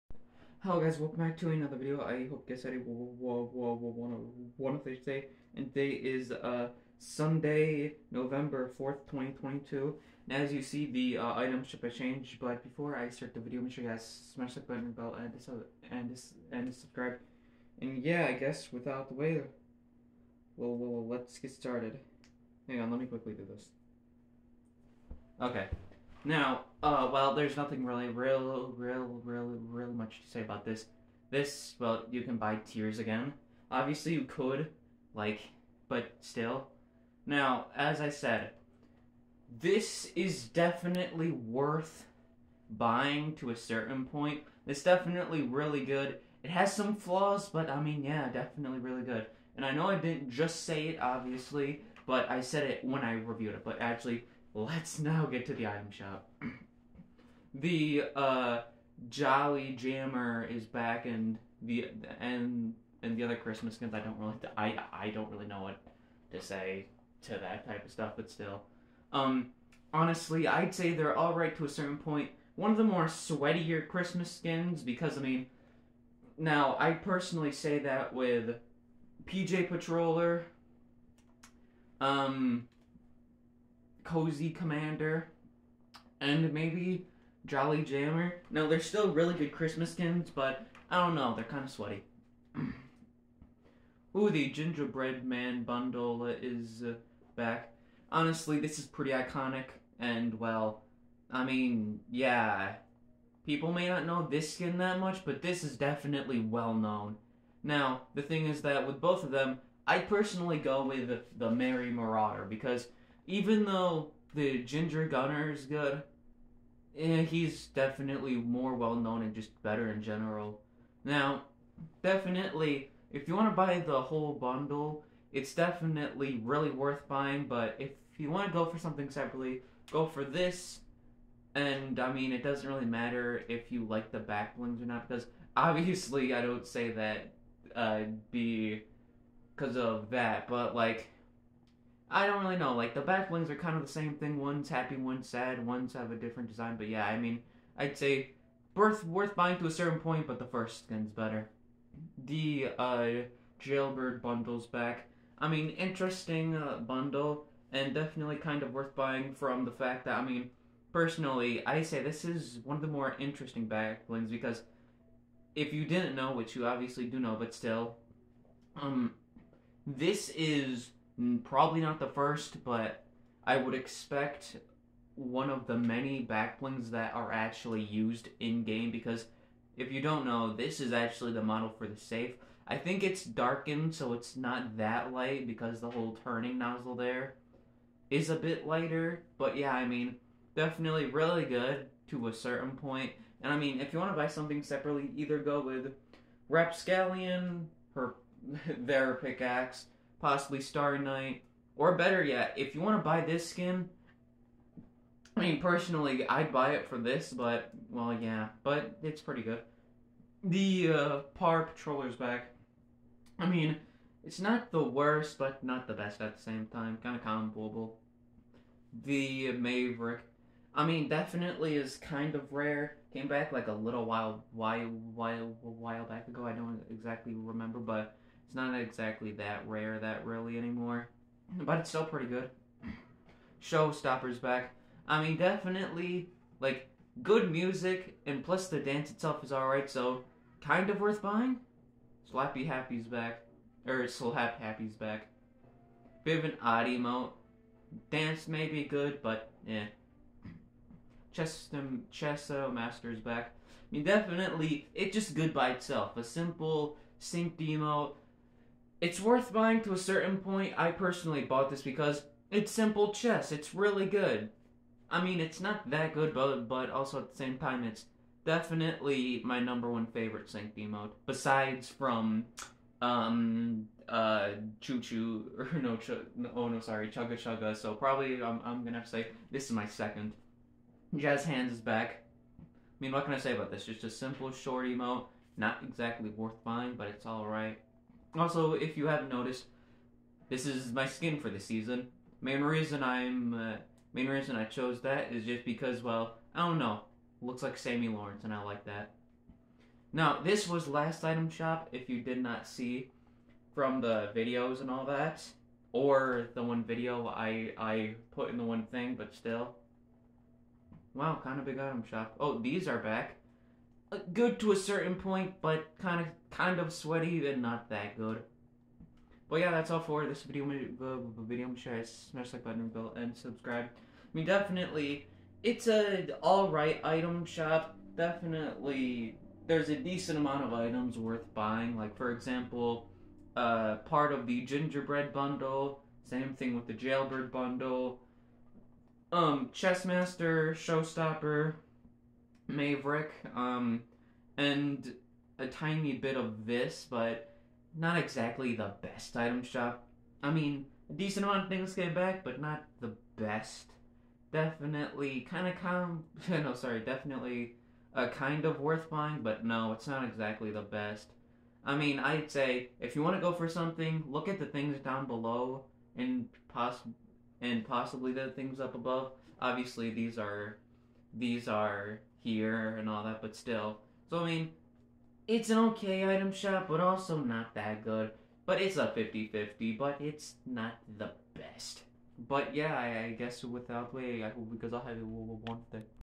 The Cozy Commander Set is back Sun Dec 4
honestly this item shop isn't that good, there's a couple items worth buying but there isn't really much really that good most of this item shop is just small Christmas stuff and simple stuff which is all right but there's definitely better items it's still not the worst thing but at the same time it's not good at all really personally I'd say, but besides that this item shop isn't exactly the best kind of good but also at the same time a good amount of it is not exactly the best it's still all right but we have had better